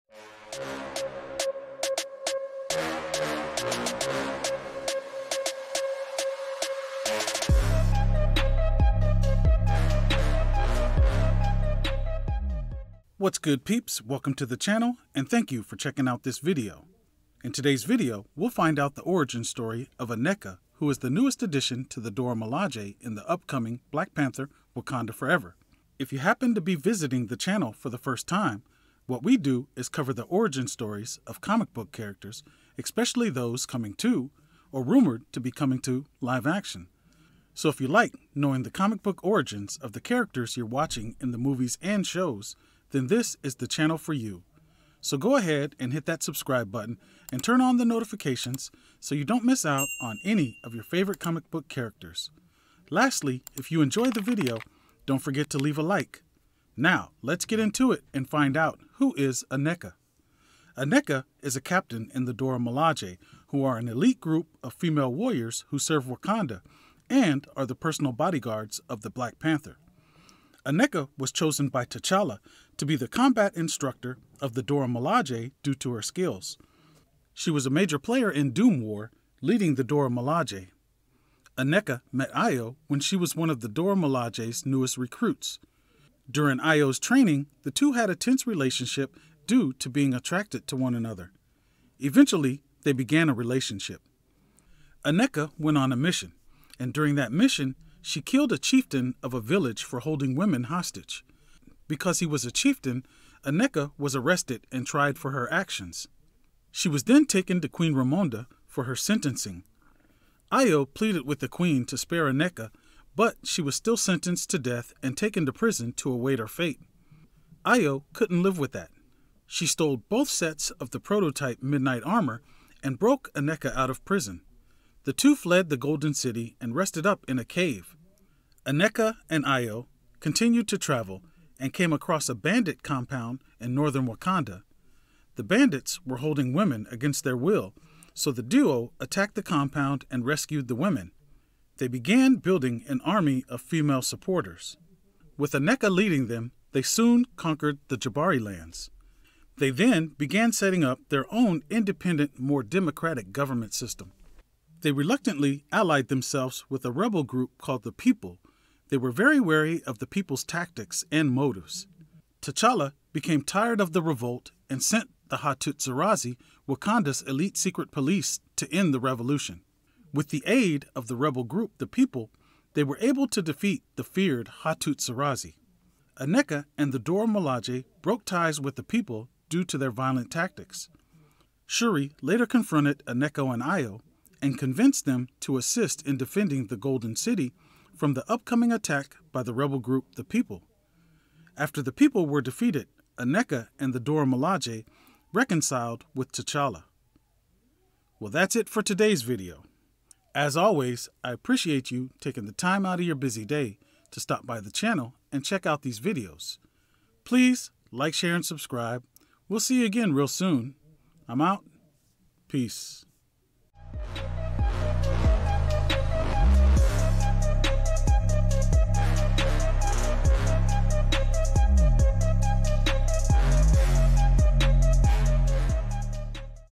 Who is Aneka? "Elite Dora Milaje member" (Marvel)
What;s good peeps? In todays video we get to know the origin of Aneka. Aneka is the newest member of the Dora Milaje in Black Panther Wakanda Forever. Why is she being introduced? Does she play a big role in the Dora Milaje? How did she come to be a member of the Midnight Angels? Join us as we find out.